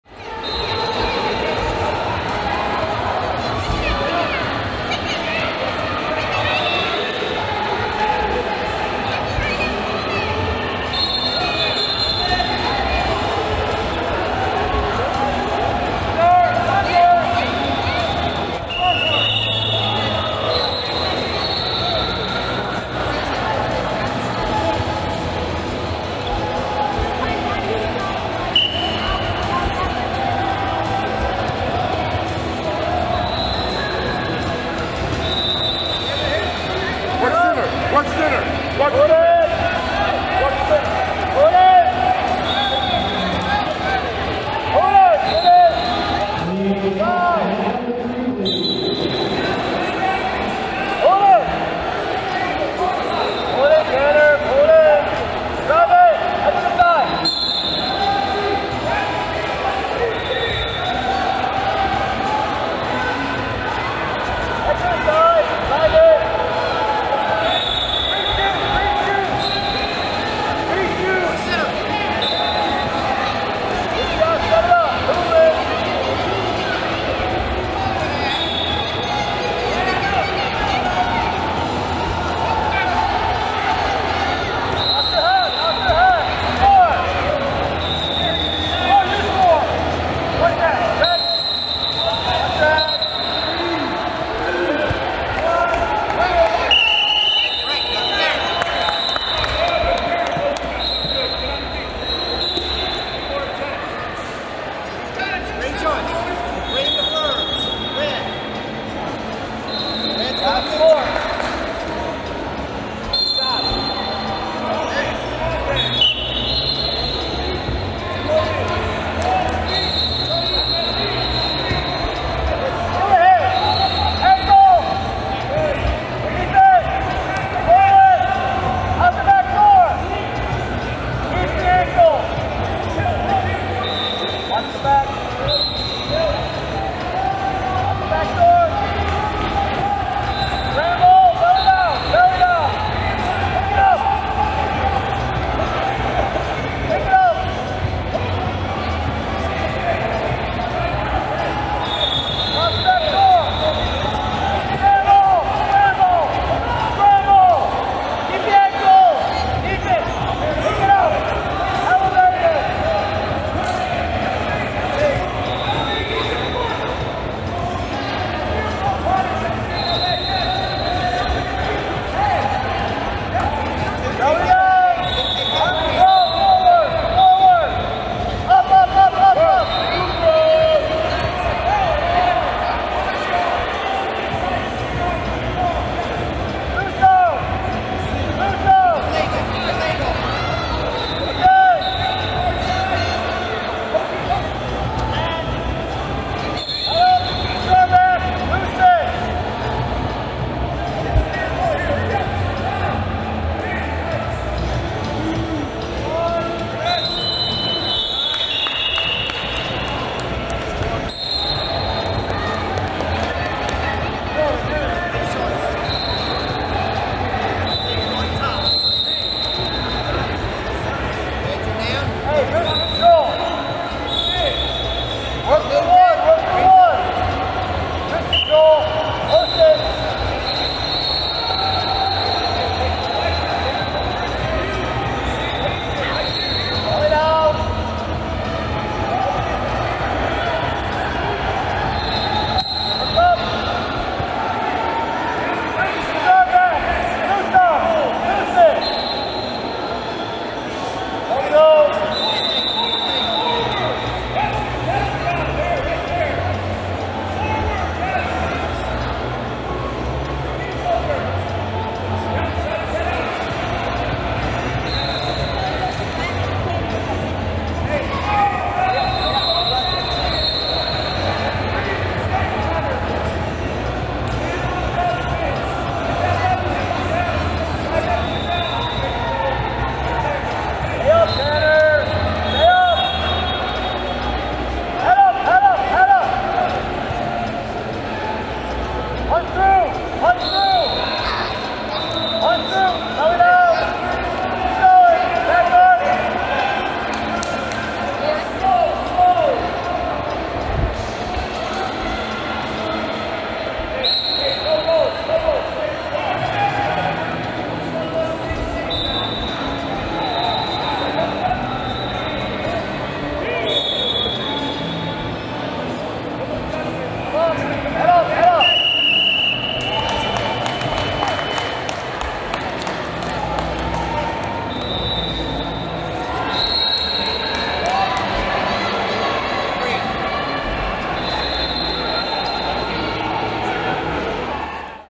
74 12U – Kaden Potter {G} of St Charles IL vs. Tanner Stone {R} of Illinois Gladiators
September 11, 2021 — RMN Strike Zone Individual Wrestling Tournament at TBK Bank Sports Complex in Bettendorf, Iowa

Kaden Potter {G} of St. Charles IL defeated Tanner Stone {R} of Illinois Gladiators by 9–0